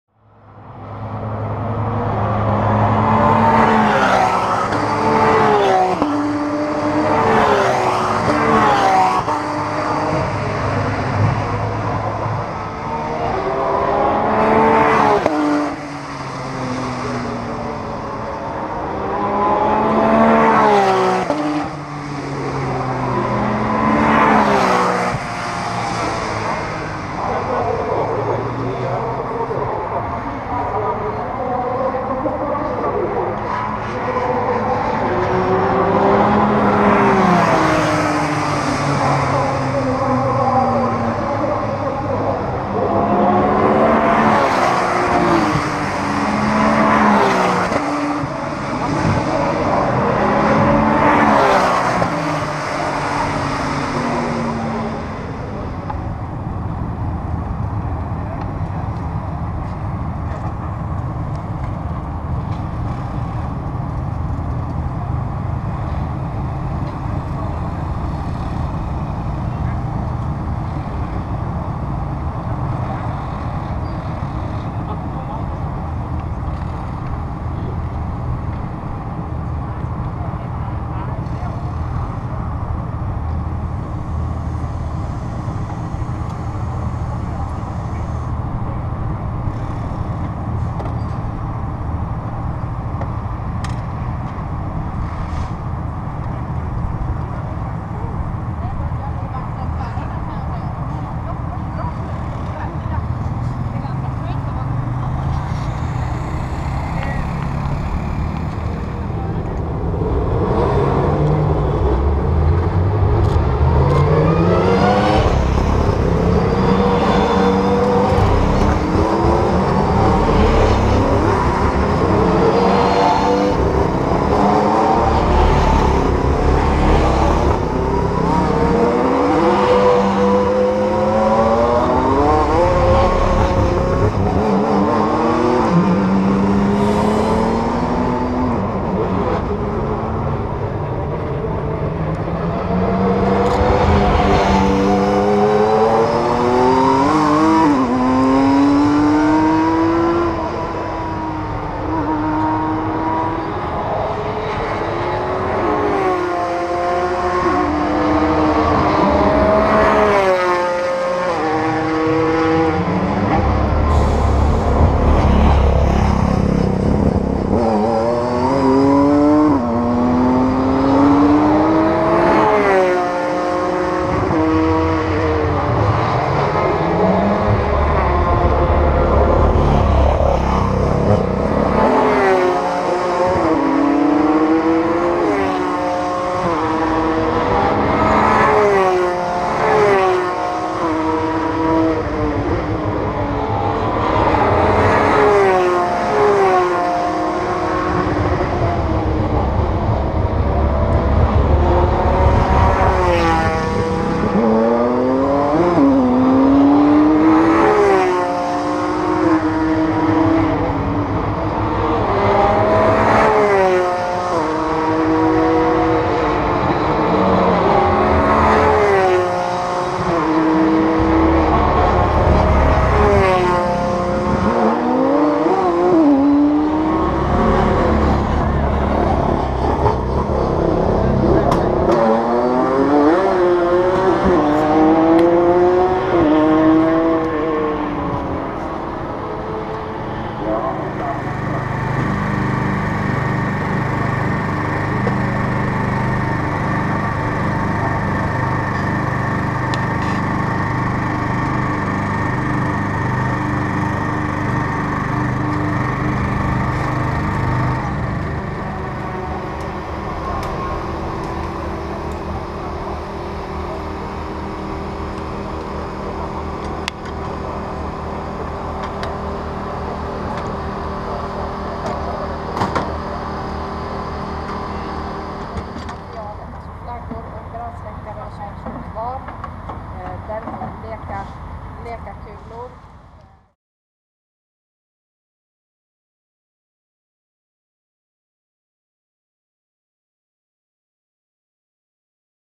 STCC finalen Mantorp Park del 7 2014 09 19
Regerande STCC-mästaren Thed Björk, Volvo Polestar Racing, hade knappast kunna skaffa sig ett bättre utgångsläge för att ta sin andra raka titel. Björk blev tvåa i det första kvalet och etta i det andra. Värste konkurrenten Fredrik Larsson, WCR/BMW Dealer Team, blev inte bättre än sjua och sexa. Nu har Björk ett försprång på 43 poäng med bara 50 poäng kvar att köra om i morgondagens två heat på Mantorp Park.